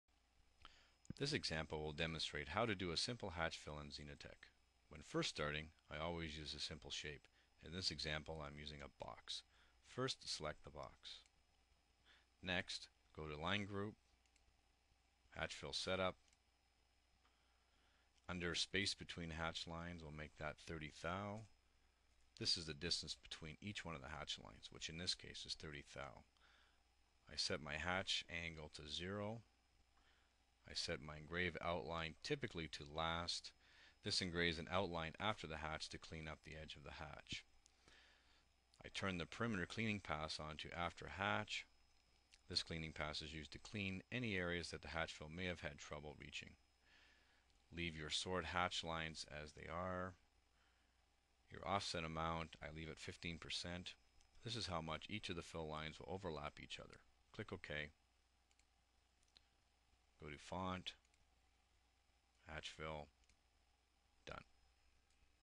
Simple Hatch Fill in Xenetech
This video shows you how to create a simple hatch pattern in an object in Xenetech 2002 Video